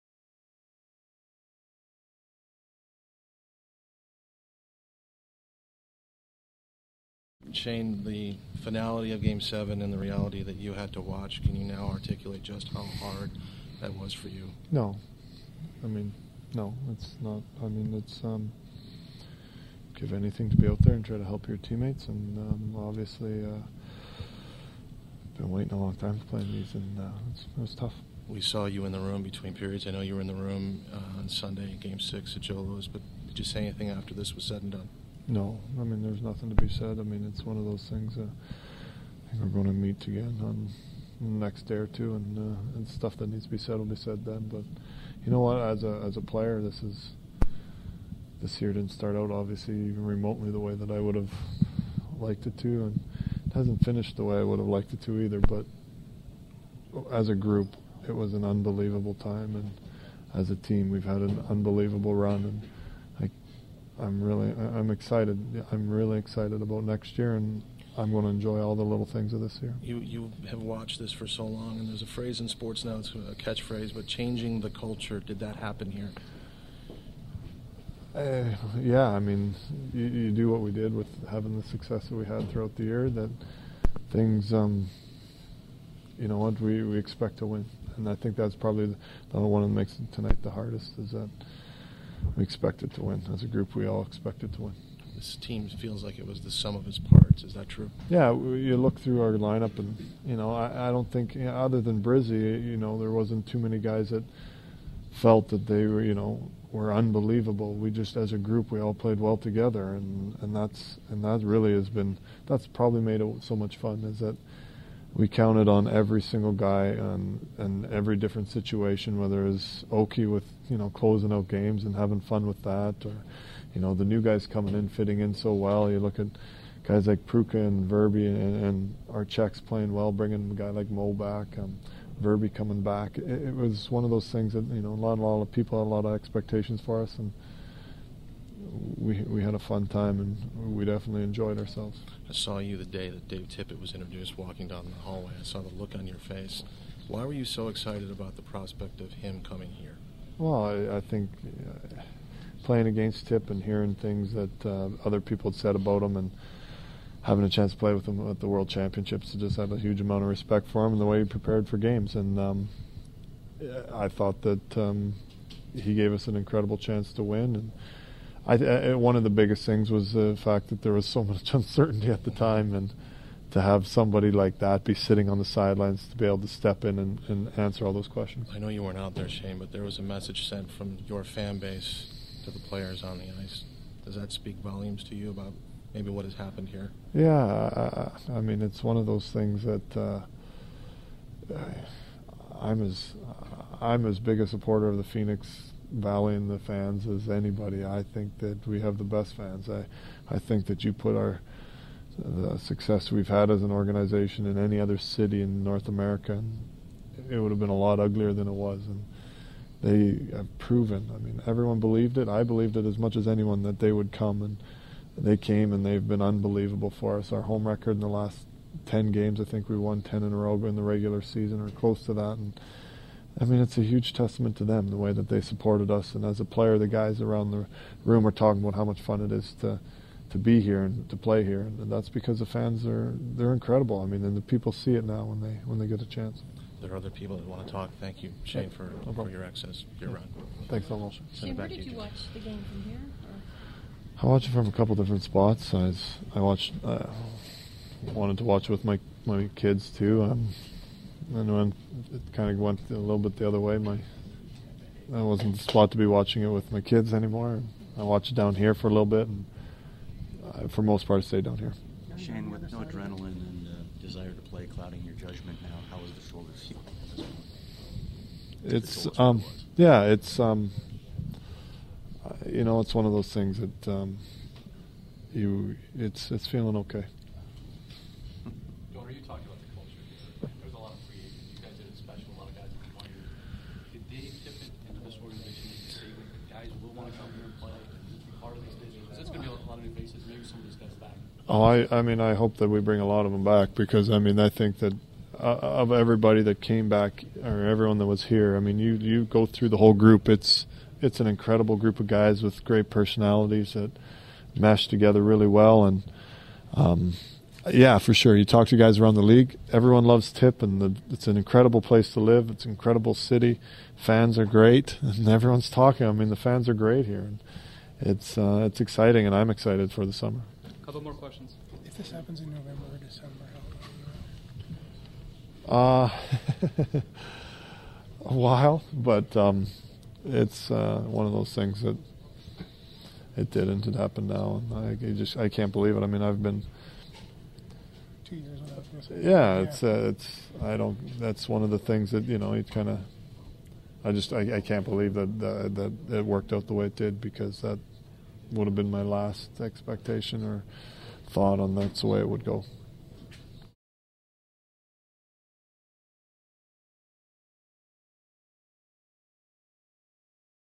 Shane Doan following Game 7
Coyotes captain Shane Doan on the Game 7 loss to Detroit and how hard it was to sit and watch most of the series with a shoulder injury.